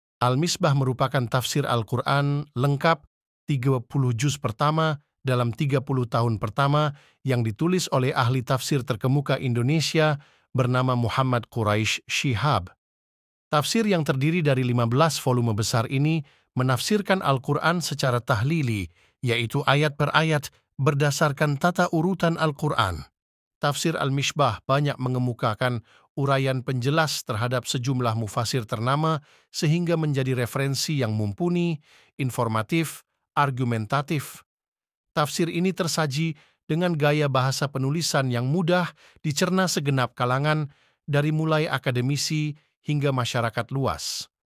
SIAP DIPESAN!!! TAFSIR AL-MISBAH ASLI LENGKAP 15 JILID KARYA QURAISH SHIHAB
Tafsir Al Misbah

Al Misbah merupakan tafsir Al Quran lengkap 30 juz pertama dalam 30 tahun pertama yang ditulis oleh ahli tafsir terkemuka Indonesia.

Keindonesiaan penulis memberi warna menarik dan khas serta sangat relevan untuk memperkaya khazanah pemahaman dan penghayatan kita terhadap rahasia makna ayat-ayat Allah. Muhammad Quraish Shihab, Lelaki yang dilahirkan di Rapang, Sulawesi Selatan ini menuangkan tafsirnya ke dalam 15 volume buku.Tafsir al-Mishbah wajah baru dilengkapi dengan navigasi rujukan silang, dan dikemas dengan bahasa yang mudah dipahami serta pengemasan yang lebih menarik.

Tafsir yang terdiri dari 15 volume besar ini menafsirkan al-Qur’an secara tahlîlî, yaitu ayat per ayat berdasarkan tata urutan al-Qur’an. Inilah yang membedakan tafsir ini dengan karya M. Quraish Shihab lainnya semisal Lentera Hati, Membumikan al-Qur’an, Wawasan al-Qur’an, Mukjizat al-Qur’an, Pengantin al-Qur’an, dan selainnya yang menggunakan pendekatan tematik (mawdhû‘î), menafsirkan ayat-ayat al-Qur’an berdasarkan topik tertentu, bukan berdasarkan tata urutannya dalam mushaf.

Ada beberapa prinsip yang dipegangi oleh M. Quraish Shihab dalam karya tafsirnya, baik tahlîlî maupun mawdhû‘î, di antaranya bahwa al-Qur’an merupakan satu kesatuan yang tak terpisahkan. Dalam al-Mishbâh, beliau tidak pernah luput dari pembahasan ilmu al-munâsabât yang tercermin dalam enam hal:
1. keserasian kata demi kata dalam satu surah;
2. keserasian kandungan ayat dengan penutup ayat (fawâshil);
3. keserasian hubungan ayat dengan ayat berikutnya;
4. keserasian uraian awal/mukadimah satu surah dengan penutupnya;
5. keserasian penutup surah dengan uraian awal/mukadimah surah sesudahnya;
6. keserasian tema surah dengan nama surah.

Tafsir al-Mishbah banyak mengemukakan ‘uraian penjelas’ terhadap sejumlah mufasir ternama sehingga menjadi referensi yang mumpuni, informatif, argumentatif.
Tafsir ini tersaji dengan gaya bahasa penulisan yang mudah dicerna segenap kalangan, dari mulai akademisi hingga masyarakat luas. Penjelasan makna sebuah ayat tertuang dengan tamsilan yang semakin menarik atensi pembaca untuk menelaahnya.

SPESIFIKASI PRODUK:
Tafsir Al Misbah lengkap (cetakan terbaru)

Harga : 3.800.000
Penulis : Prof. Dr. Quraish Shihab
Penerbit : Lentera Hati
Ukuran : 16 x 24 cm
Tebal : 10.714 halaman dlm 5 jilid
Kertas : HVS putih
Sampul : hard cover
Berat : 20 kg

Jumlah Halaman : 15 Jilid
Jilid 1: 788 hlm Jilid 6: 790 hlm Jilid 11: 688 hlm
Jilid 2: 854 hlm Jilid 7: 726 hlm Jilid 12: 638 hlm
Jilid 3: 780 hlm Jilid 8: 632 hlm Jilid 13: 622 hlm
Jilid 4: 632 hlm Jilid 9: 700 hlm Jilid 14: 628 hlm
Jilid 5: 802 hlm Jilid 10: 664 hlm Jilid 15: 770 hlm

(Bapak Zain)

quraish shihab tafsir al misbah full,